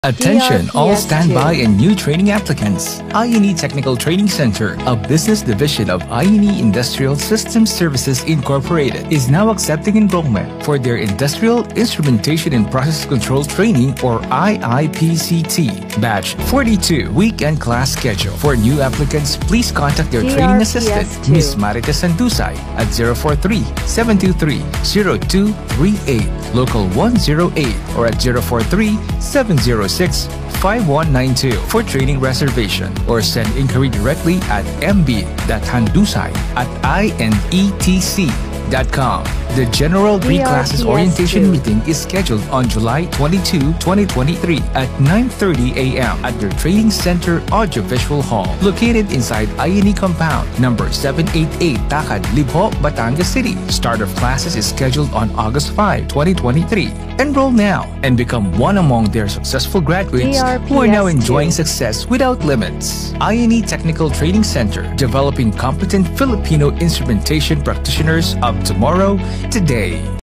I&E Technical Training Center Radio Ad Q3 2023 70s (Batangas)
Ito po ang Q3 2023 Provincial Radio Ad ng I&E Technical Training Center sa Batangas.

DRPS Two 4717: I&E Technical Training Center Radio Ad Q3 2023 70s (Batangas)